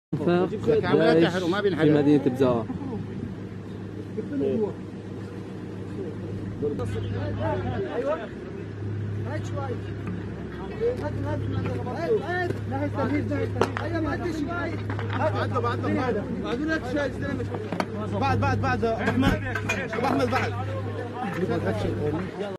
FSA found and plugged a sewer/water evacuation used by ISIS to move fighters/weapons on Bzaah front.
Bizaa kapasında deaş tüneller kullanarak bölgede bulunan Öso savaşçılarına saldırı yaptığı tüneller..
Öso tünelleri kapatıyor..